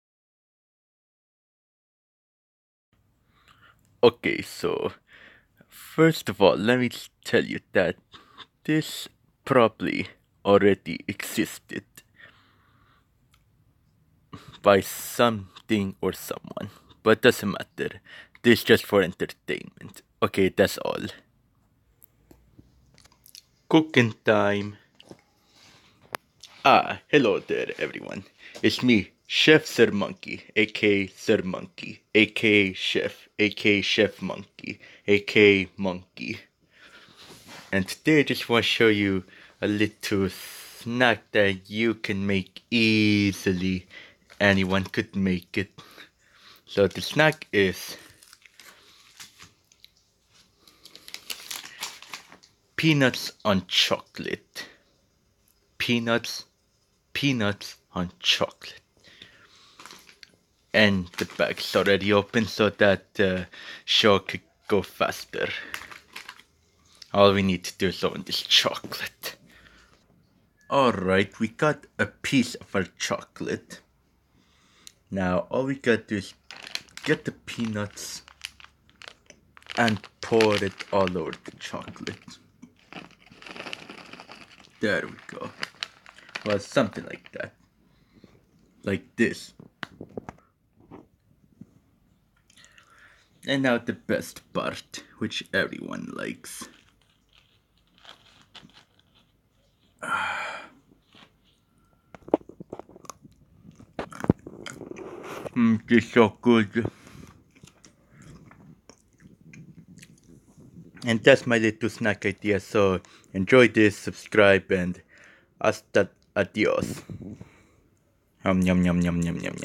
Monki Idea: Peanut Chocolate
An fun idea of a creative snack. This probably exists, byt doesn't matter cuz this is for entertainment.